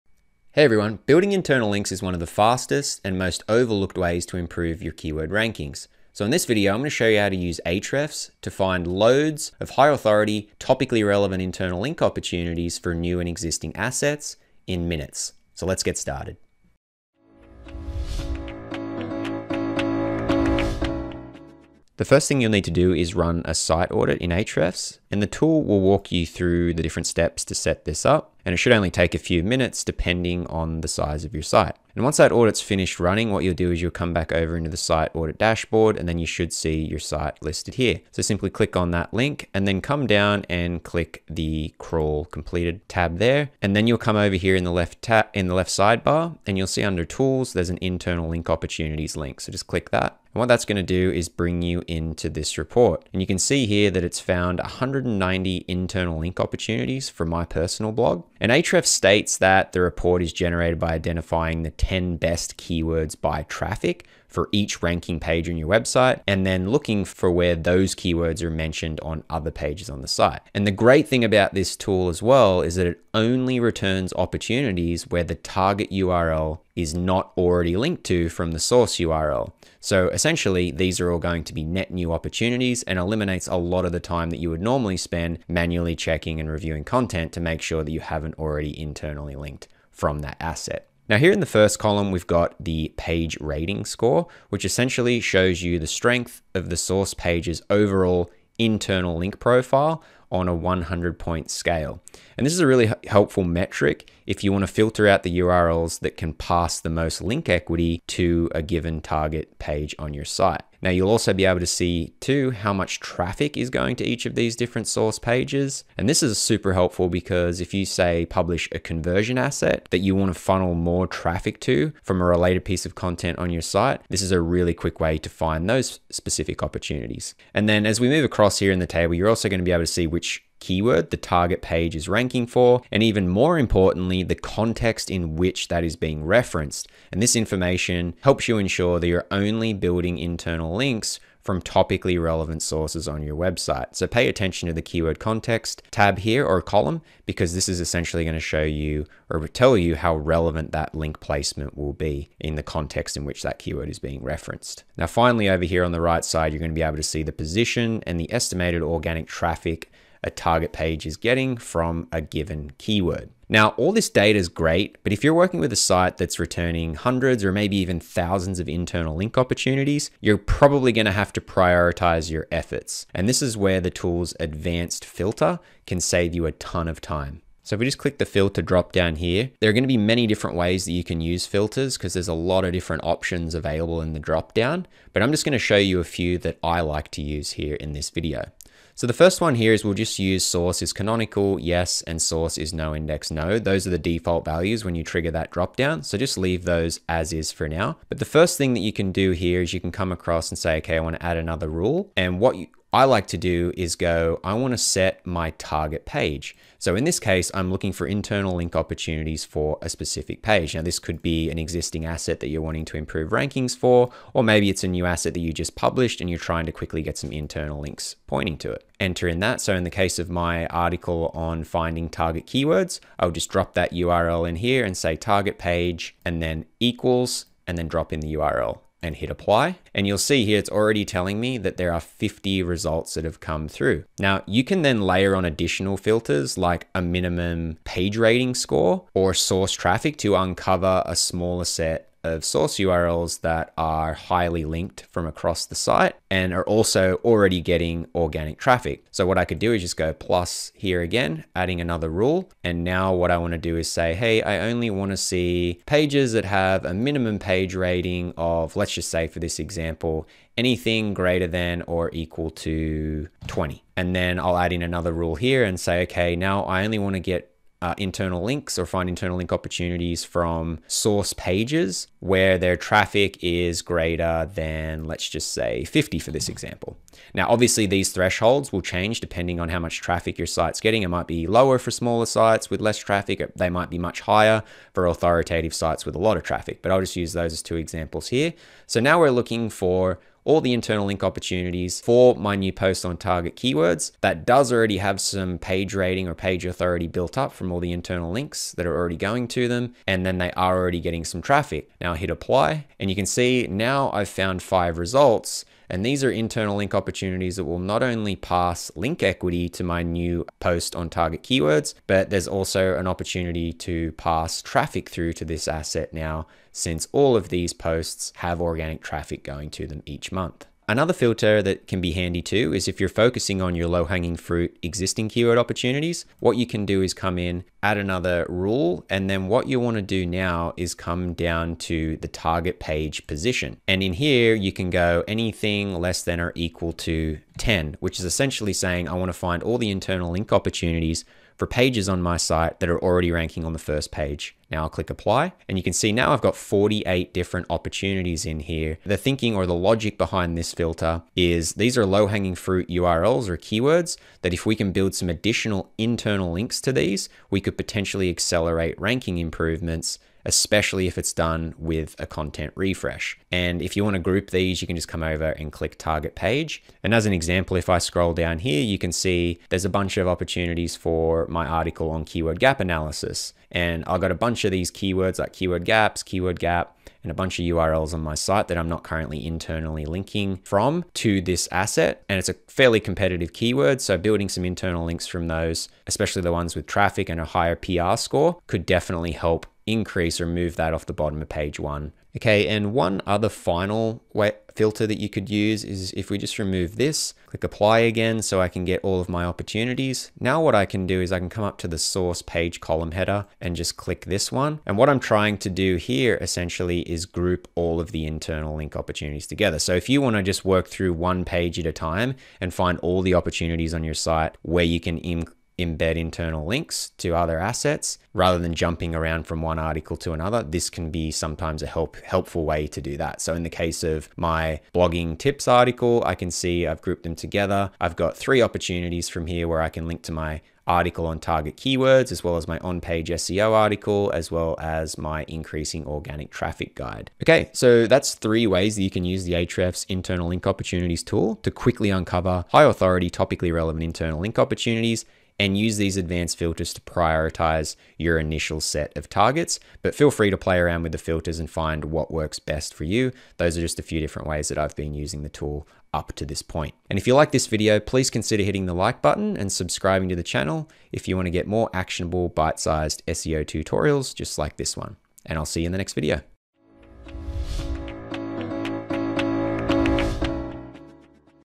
How to Find Quality Internal Link Opportunities (With Ahrefs)
Building internal links is one of the fastest and most overlooked ways to improve your keyword rankings.

In this video tutorial, I'll show you how to use several filters within the Ahrefs Internal Link Opportunities report to quickly find high-quality topically-relevant internal link opportunities for your content. This will save you a TON of time!

➡️  RESOURCES:

👉 Join 700+ other students and get access to my premium training program, The SEO Playbook